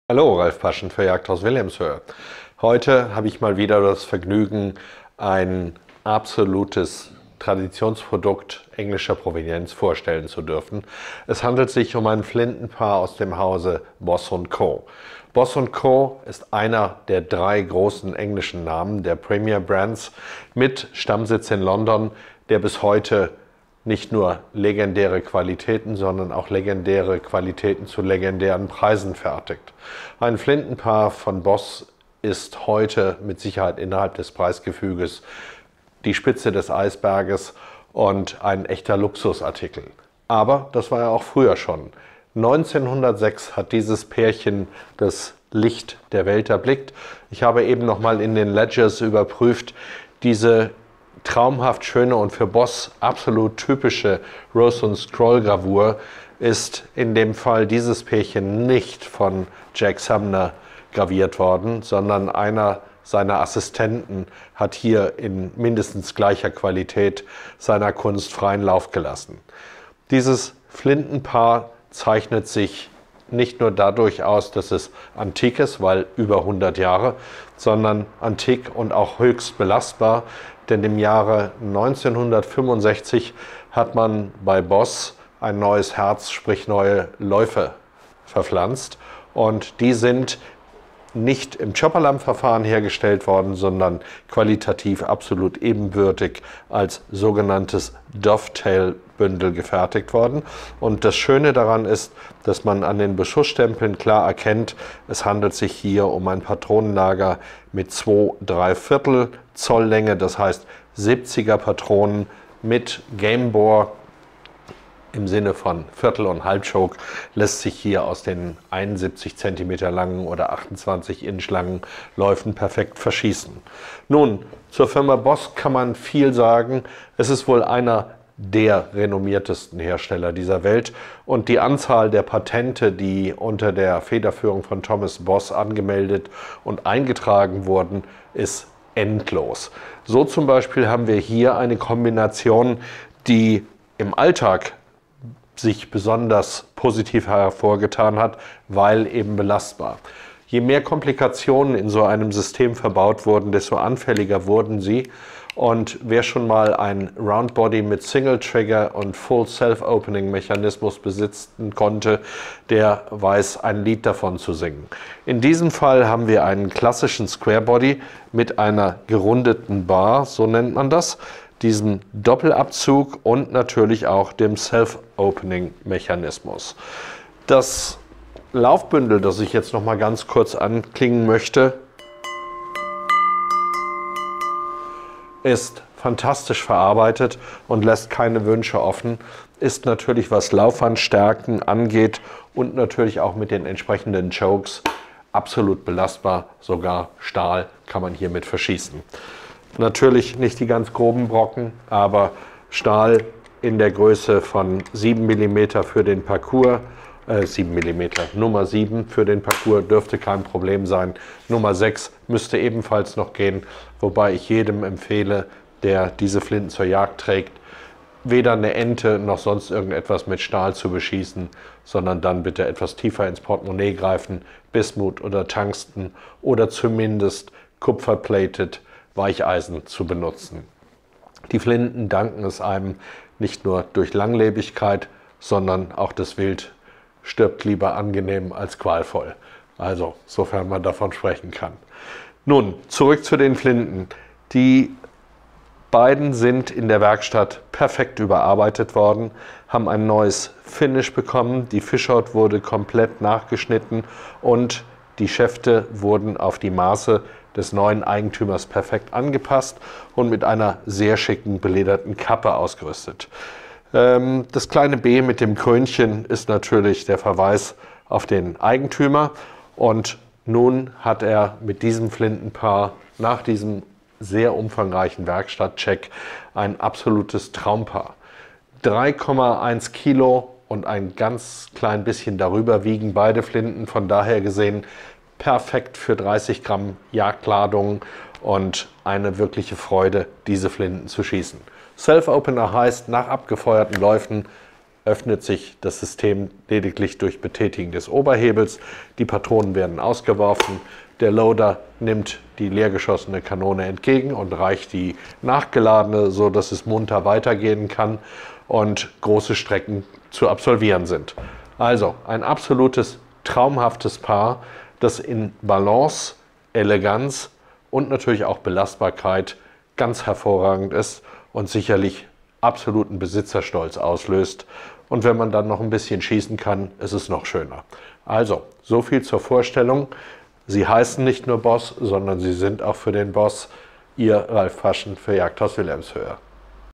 Ralph Paschen für Jagdhaus Wilhelmshöhe: Klassische Eleganz - Pärchen von Boss & Co im Kaliber 12
Ich freue mich, Ihnen ein exquisites Paar Schrotflinten von Boss & Co vorzustellen, gefertigt in der renommierten Tradition feinster englischer Büchsenmacherkunst. Dieses Paar im Kaliber 12 zeichnet sich durch seine herausragende Verarbeitung und die unverwechselbare Balance aus, die Kenner so sehr schätzen. Die Flinten verfügen über fein gravierte Stahlbaskülen und hochwertige Walnussholzschäfte, die eine beeindruckende Ästhetik bieten. Komplettiert wird dieses Set durch einen maßgefertigten Koffer, der nicht nur Schutz bietet, sondern auch dem hohen Standard dieser Waffen entspricht. In unserem Video geben wir Ihnen detaillierte Einblicke in die Besonderheiten dieser bemerkenswerten Flinten und erläutern, was sie zu einer wahren Investition für Sammler und Schützen macht.

Mit Waidmannsheil
Ihr
Ralph Paschen